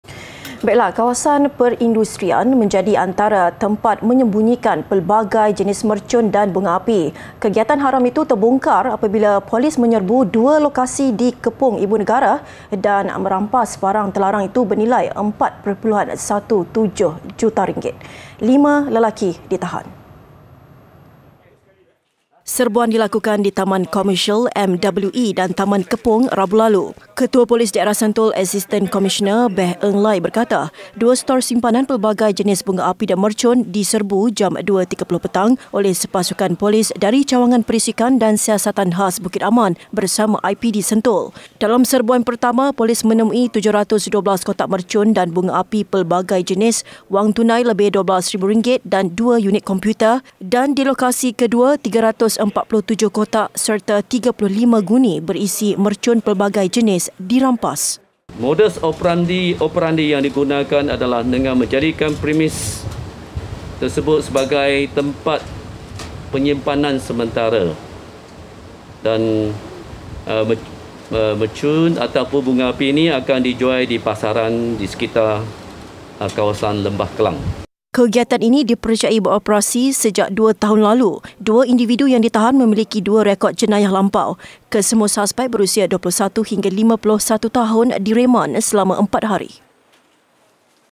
Sorok Mercun & Bunga Api Di Kawasan Perindustrian
Kawasan perindustrian menjadi antara tempat menyembunyikan pelbagai jenis mercun dan bunga api.

Kegiatan haram itu terbongkar apabila polis menyerbu dua lokasi di Kepong ibu negara dan merampas barang terlarang itu bernilai RM4.17 juta.

Lima lelaki ditahan.